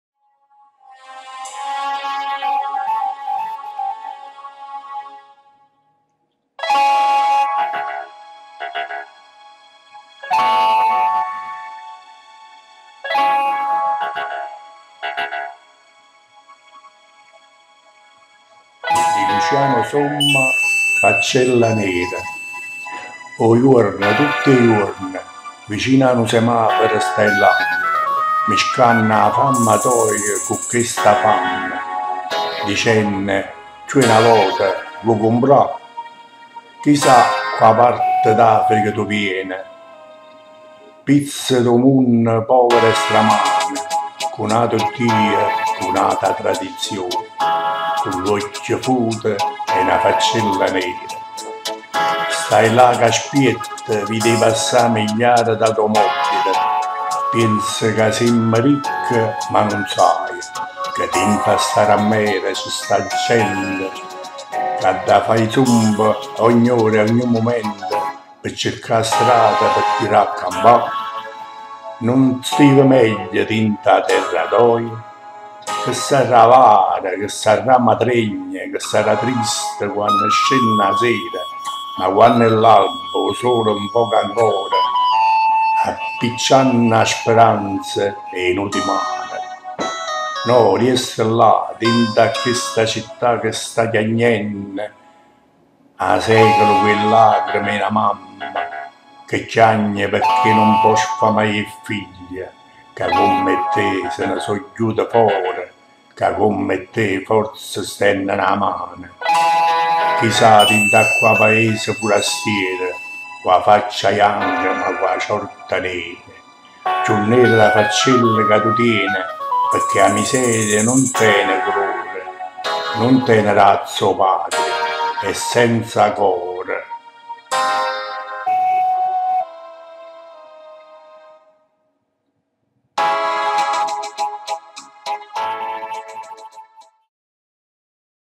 "Faccella nera" 1
Una poesia di Luciano Somma
poeta e scrittore napoletano